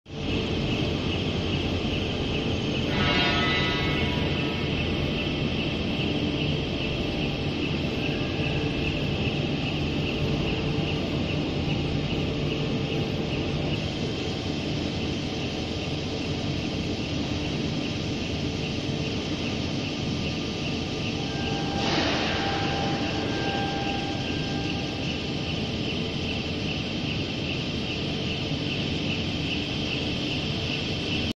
Coil Transfer Trolley-Rail Transfer Trolleys
The coil transfer trolley is powered by low voltage rail. It has the following advantages: full automatic, easy operation, simple structure and low price.